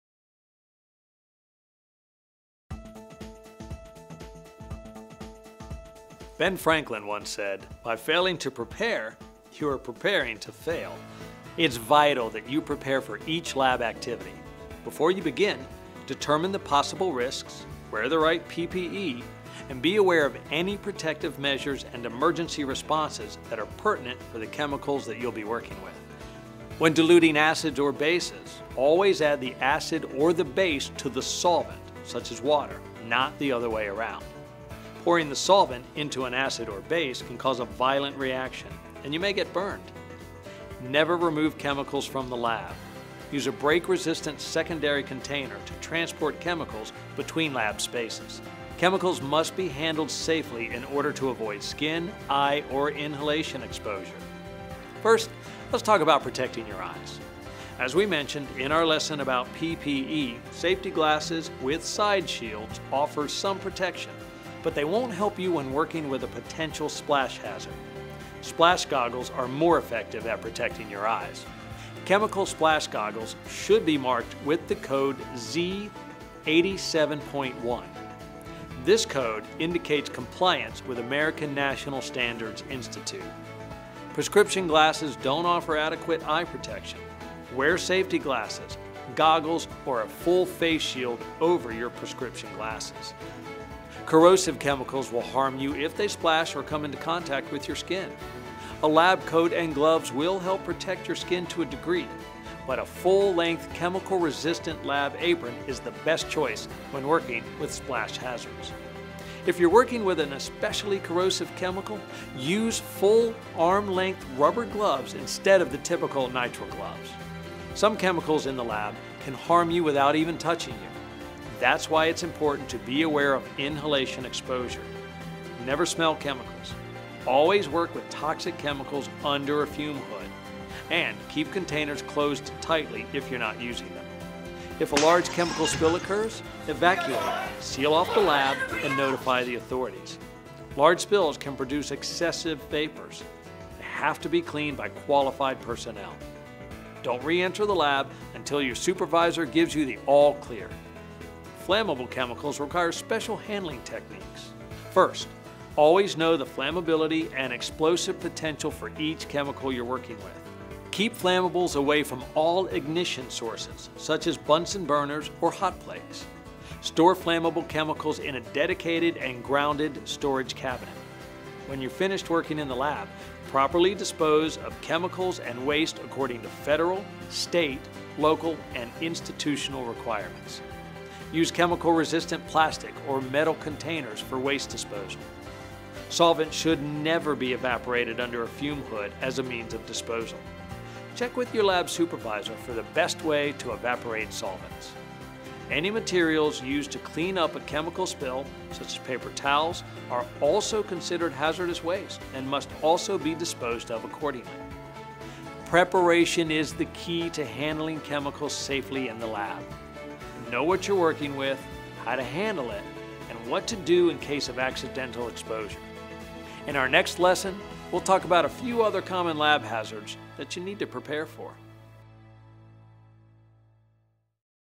Safe Chemical Handling / Lab Safety Video Part 5
It's vital that you prepare for each lab activity by determining the possible risks, donning the right PPE, and be aware of any protective measures and emergency responses that are pertinent for the chemicals that you'll be working with.
•  Eye protection
•  Transporting chemicals
•  Lab aprons and arm-length rubber gloves
•  Inhalation exposure
•  Chemical spill response
•  Flammables
•  Chemical disposal

Check out our exciting short film, "Zombie College: The 5 Rules of Lab Safety," available now

Hosted by Tim Ross
Special thanks to:
Amber Baker
Chase Turner
Chris Puente
Dan McClellan
Diane Hendrickson
Duane Adams
Harry Schultz
Jamie Costa
Jeff Irvin
Joe Issa
Laurel Vaughn
Marie Knight
Meghan Davis
Michael Gibbons
Mike Gentilini, Jr.
Randy Durren
Richard Chadwick
Richard Gualandi
Ryan Atkins
Sarah Schober
Summer Cortinas
Vernon Shoaf

This video provides guidance for working safely in a modern science lab and is intended to supplement the formal instruction provided in each institution and lab.  This video is not a substitute for a safety training program.